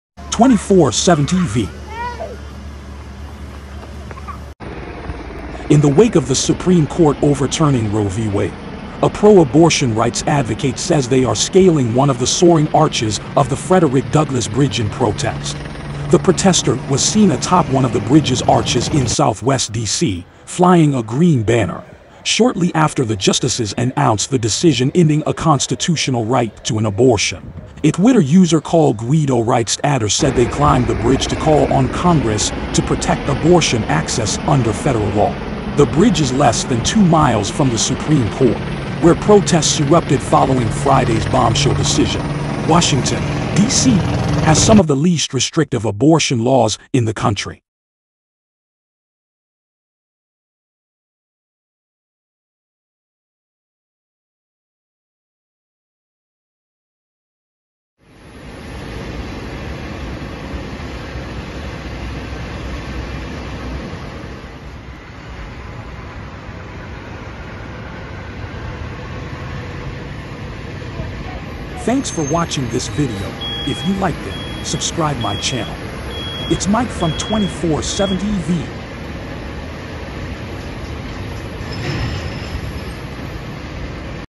Pro-Abortion-Rights Protester Climbs Frederick Douglass Bridge in DC
247TV | In the wake of the Supreme Court overturning Roe v. Wade, a pro-abortion-rights advocate says they are scaling one of the soaring arches of the Frederick Douglass Bridge in protest.
The protester was seen atop one of the bridge's arches in Southwest D.C., flying a green banner, shortly after the justices announced the decision ending a Constitutional right to an abortion.
A Twitter user called Guido Reichstadter said they climbed the bridge to call on Congress to protect abortion access under federal law.

The bridge is less than two miles from the Supreme Court, where protests erupted following Friday's bombshell decision.
Washington, D.C., has some of the least restrictive abortion laws in the country.